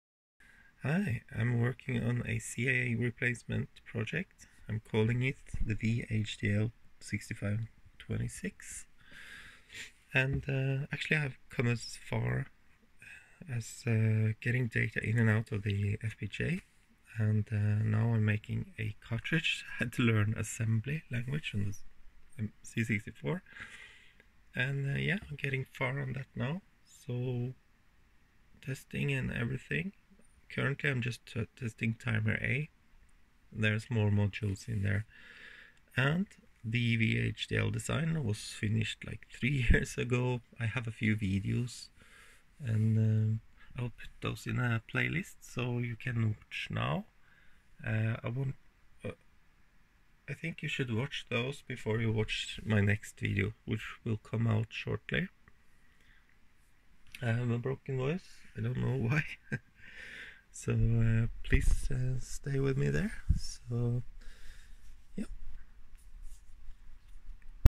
The C64 VHDL6526 project - upcoming videos
In the upcoming videos I have worked on the CIA IC replacement for the C64. I have photage in the form of worklogs.